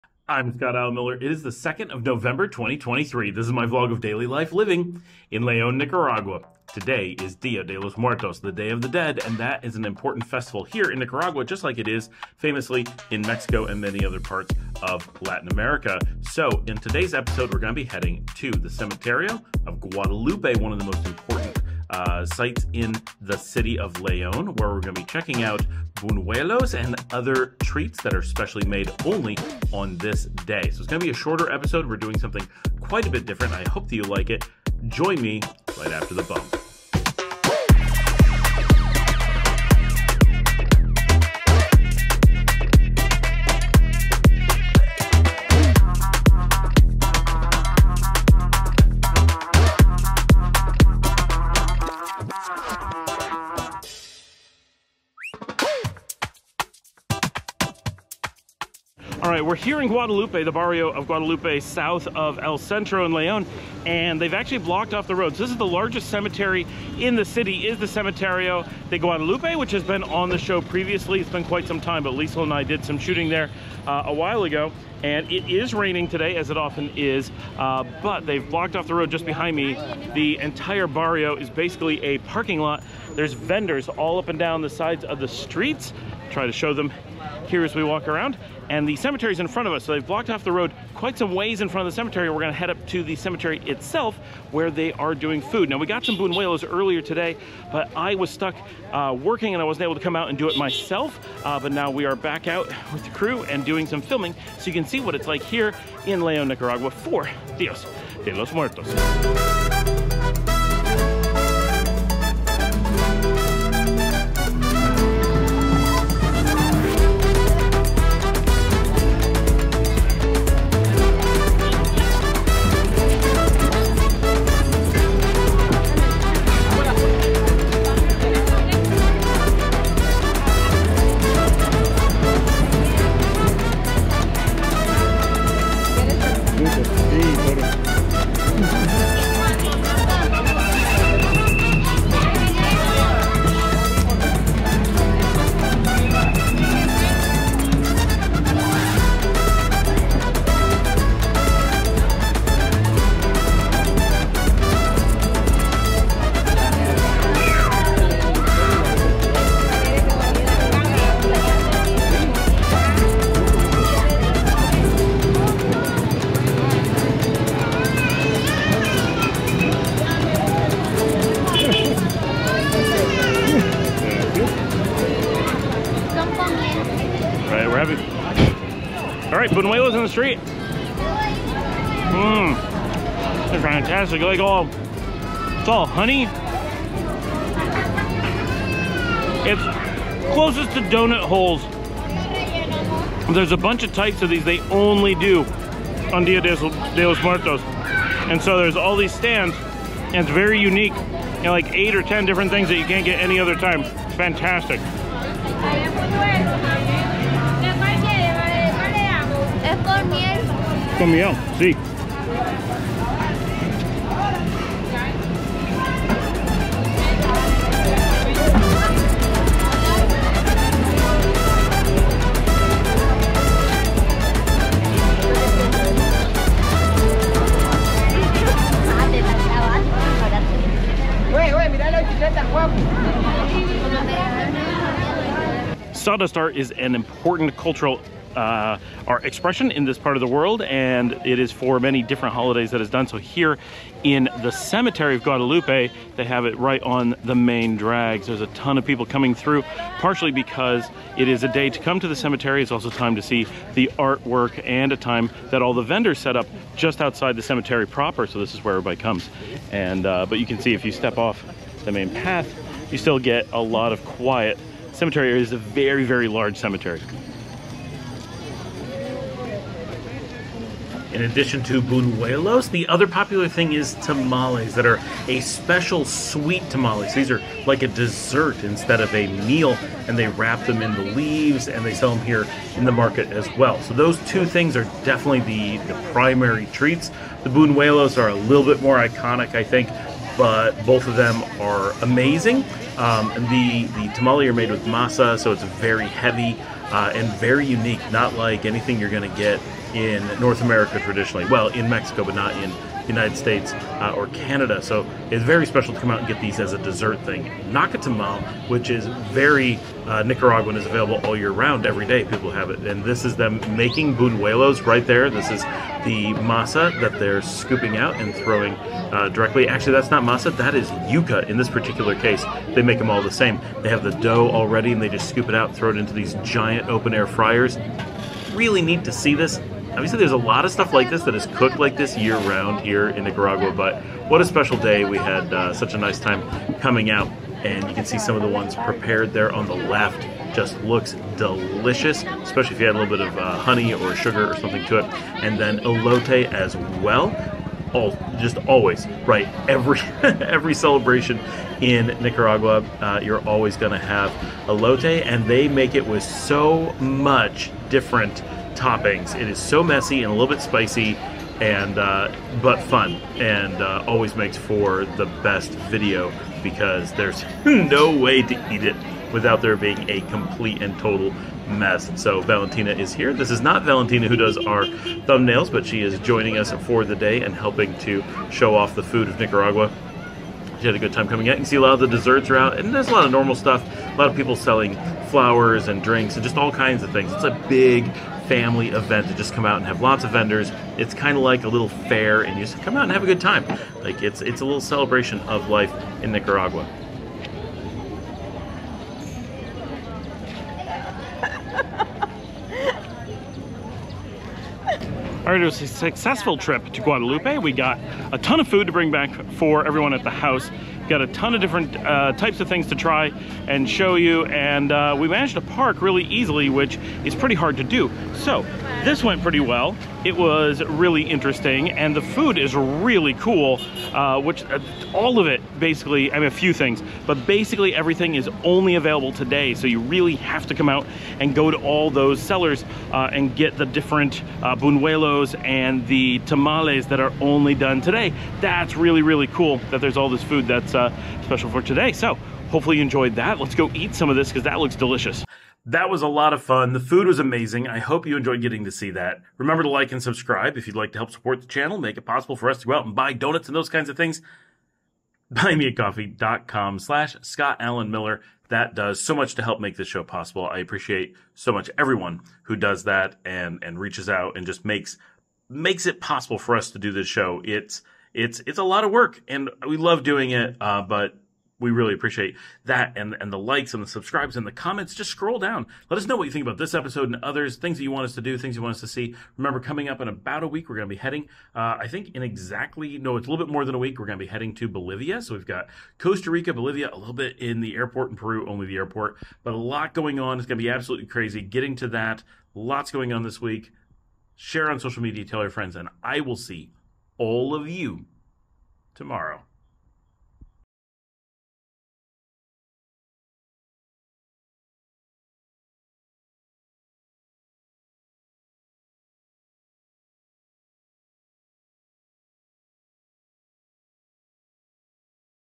Day of the Dead 🇳🇮 Leon Nicaragua Dia de Muertos
Vlog 2 November 2023 | Today is diademuertos in Nicaragua and we head to the main cemetery in leonnicaragua leónnicaragua to get buñuelos and tamales to celebrate. díademuertos diademuertos2023 dayofthedead Cemetario Guadalupe

Shot in Leon's largest cemetery which is in the Barrio of Guadalupe.

My Book on Linux Administration